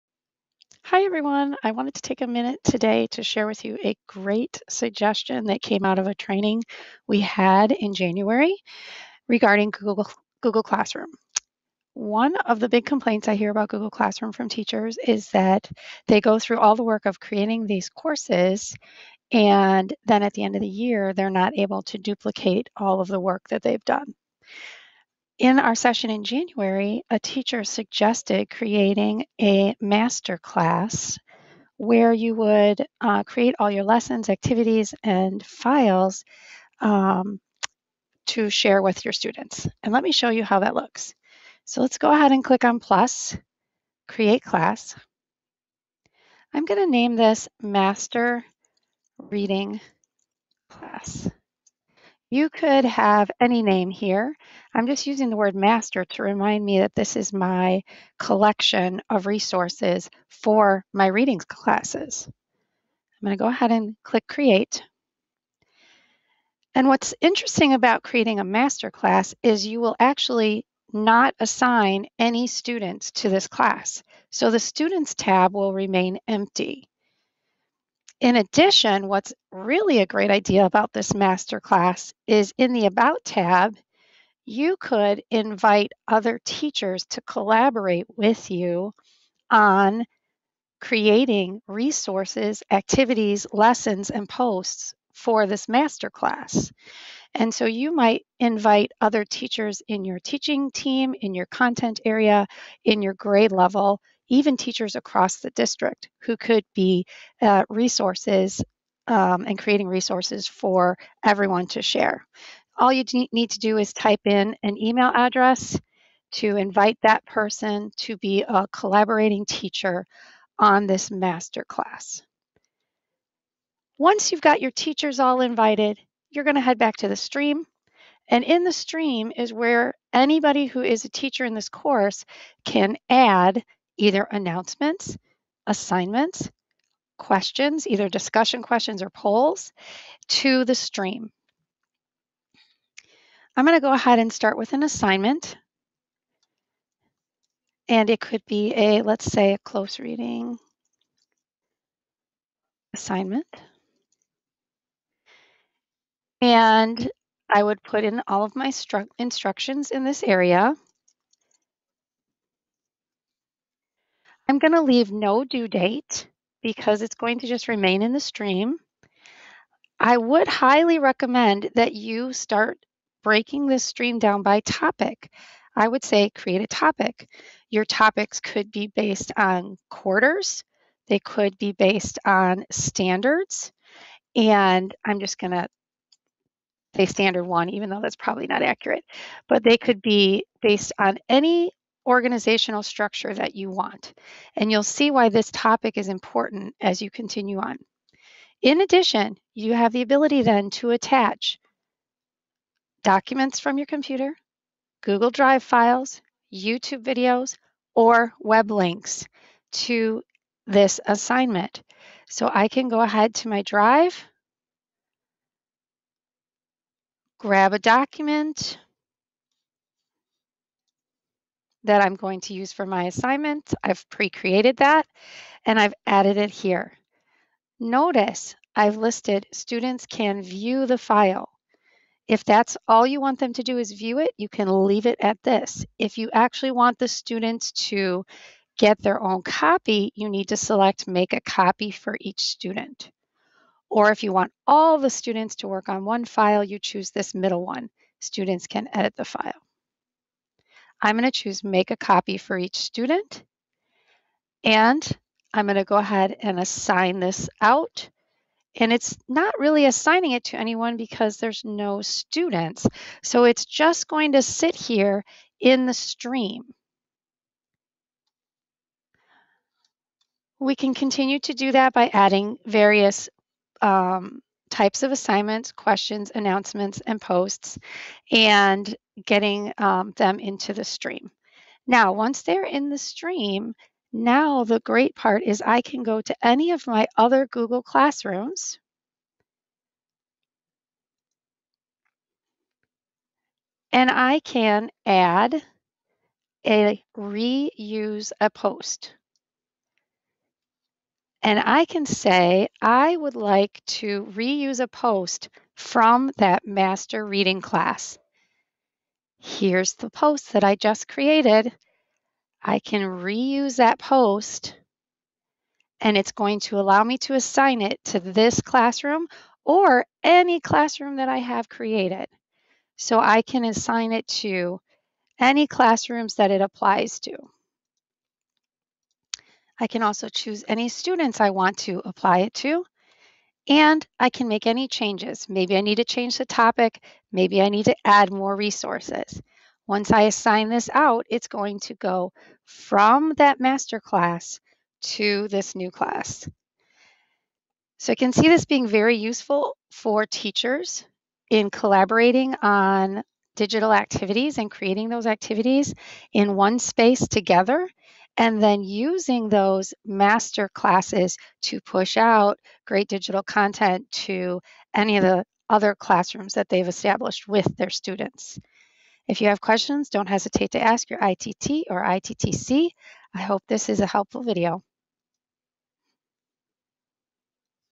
Master Classes in Google Classroom
Learn how to create a Master class of assignments, announcements and resources that can be pushed to any of your other Google Classrooms!